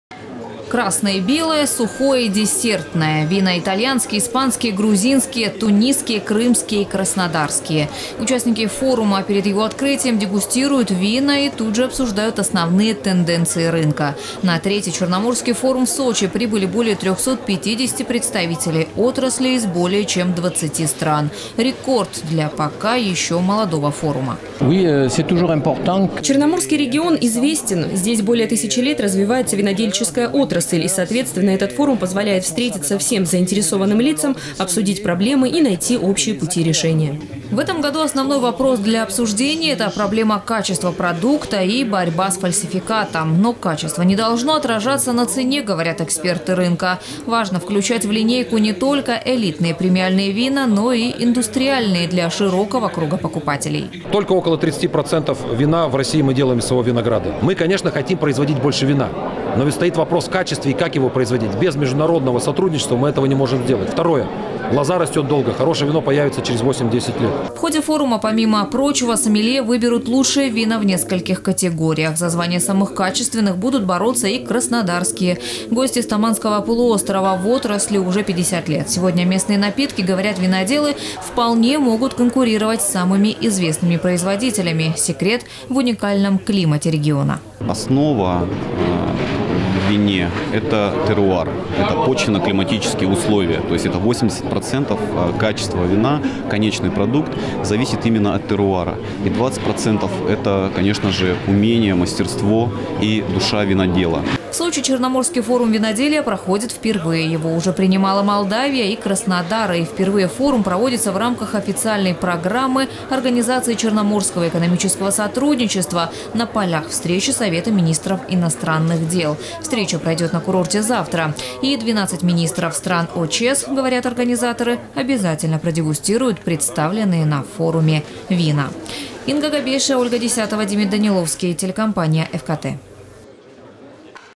В Сочи открылся III Черноморский форум виноделия Новости Эфкате Сочи
Конференция будет сфокусирована на обсуждении прогнозов и анализа основных тенденций на рынке винодельческого сектора экономики. Участники — а их в этом году более 350 — будут обсуждать и перспективные тренды международного отраслевого сотрудничества в Черноморском регионе.

Красное и белое, сухое и десертное. Вина итальянские, испанские, грузинские, тунисские, крымские и краснодарские. Участники форума перед его открытием дегустируют вина и тут же обсуждают основные тенденции рынка. На третий Черноморский форум в Сочи прибыли более 350 представителей отрасли из более чем 20 стран. Рекорд для пока ещё молодого форума.

Сочи. Новости.
Телеканал Эфкате.